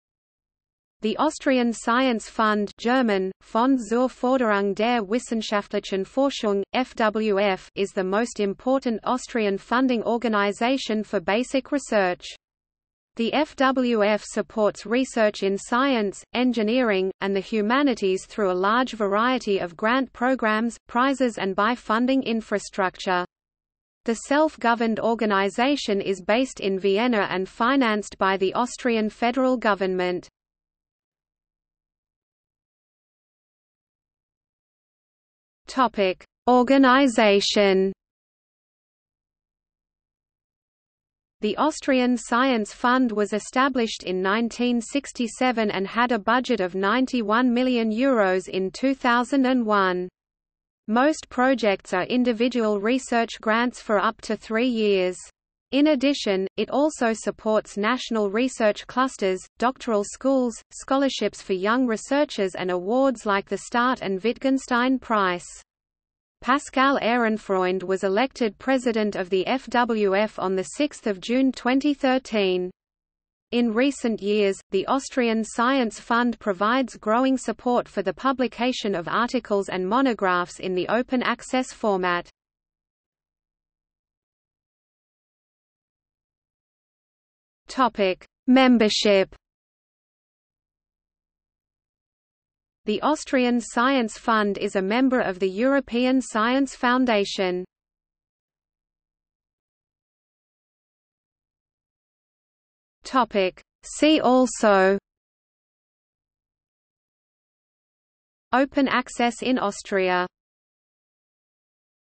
Austrian Science Fund | Wikipedia audio article
00:00:32 1 Organisation
00:01:24 2 Membership
00:01:39 3 See also

- Socrates

SUMMARY
The Austrian Science Fund (German: Fonds zur Förderung der wissenschaftlichen Forschung, FWF) is the most important Austrian funding organization for basic research. The FWF supports research in science, engineering, and the humanities through a large variety of grant programmes, prizes and by funding infrastructure. The self-governed organization is based in Vienna and financed by the Austrian federal government.